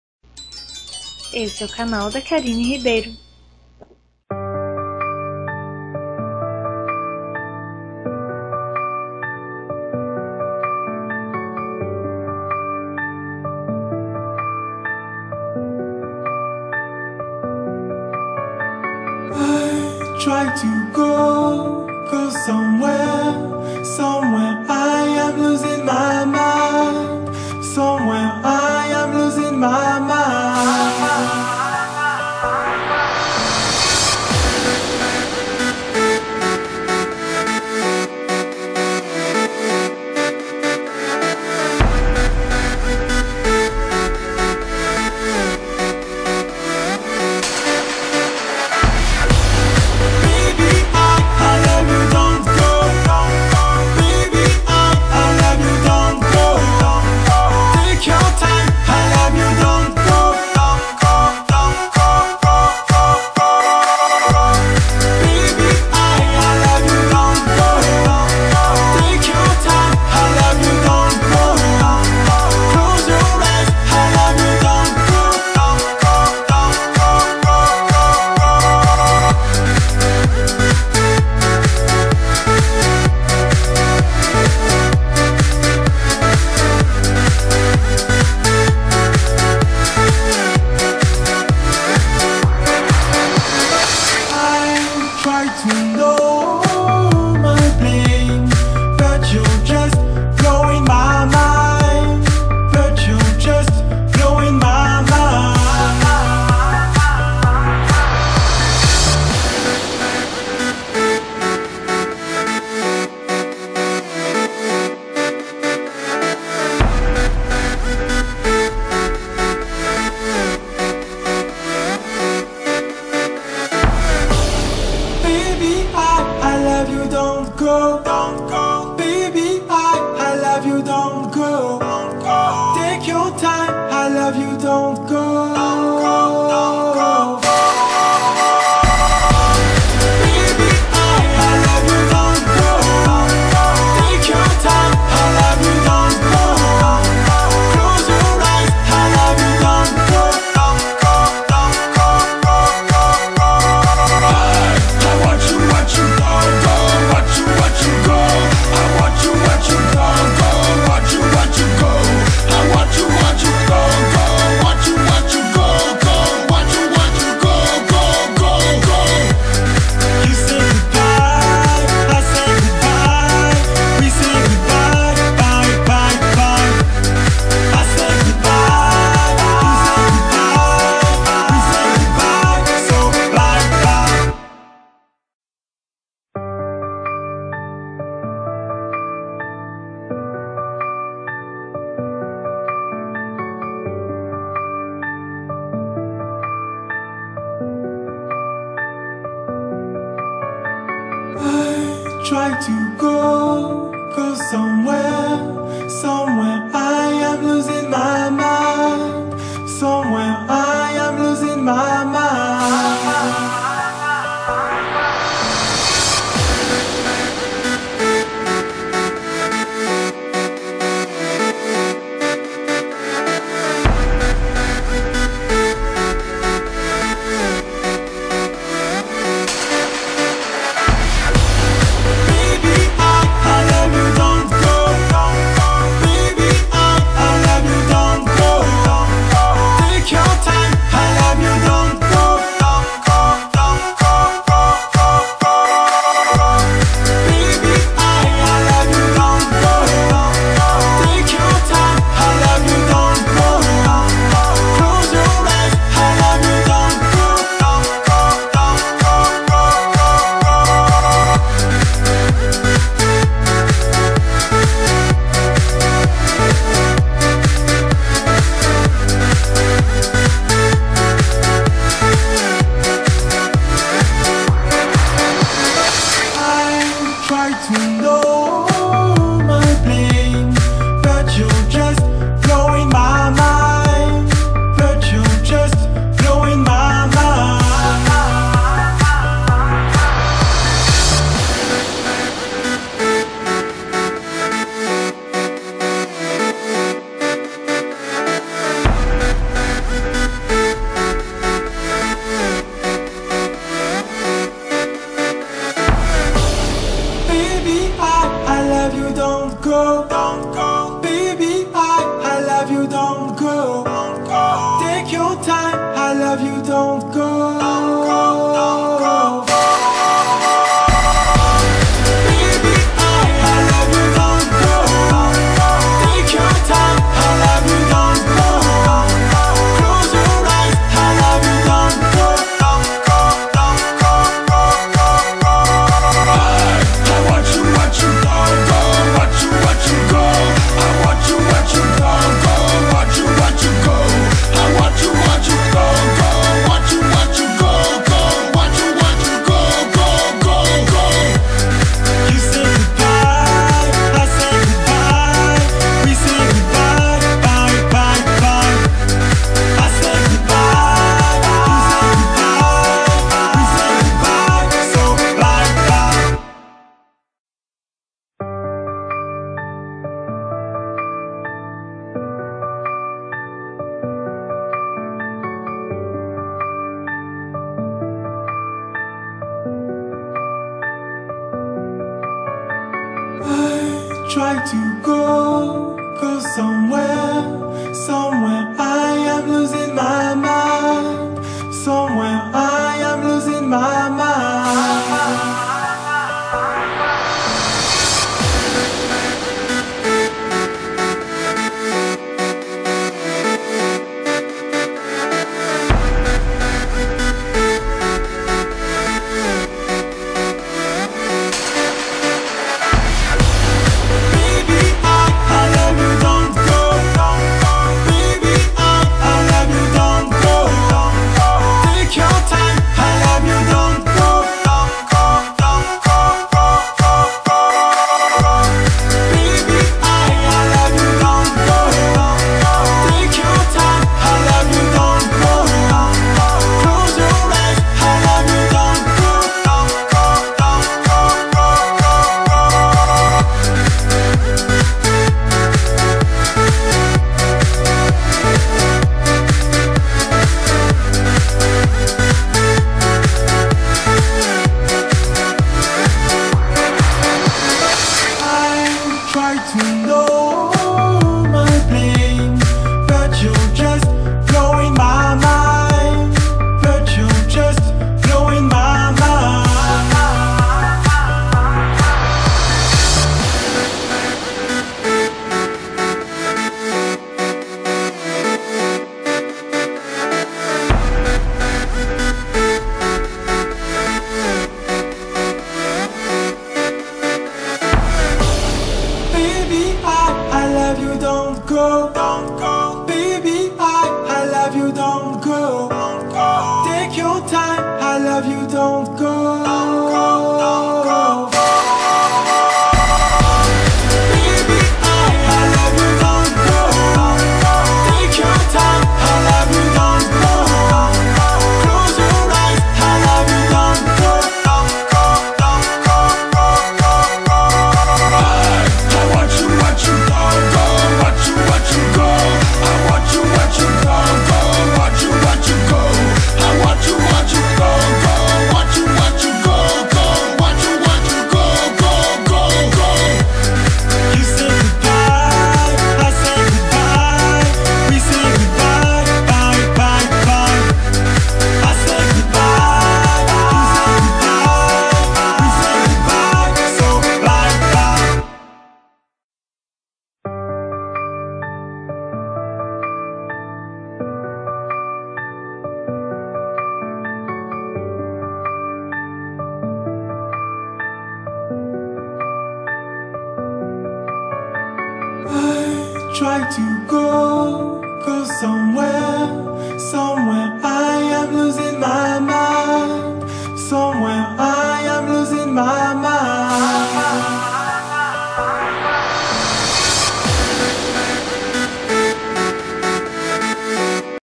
Make - Cinza e Preta - Outono Inverno - Karinne Ribeiro
Começando o frio, para minha tristeza, fiz uma make cinza e preta! espero que gostem! bjos!